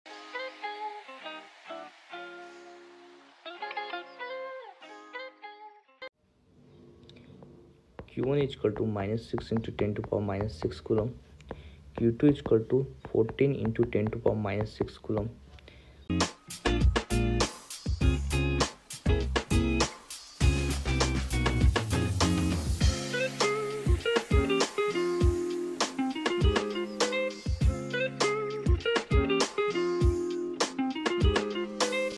Find the force of attraction between two ball bearings with charges of -6 microcoulombs and positiv…
Find the force of attraction between two ball bearings with charges of -6 microcoulombs and positive 14 microcoulombs that are 4 millimeters apart from each other.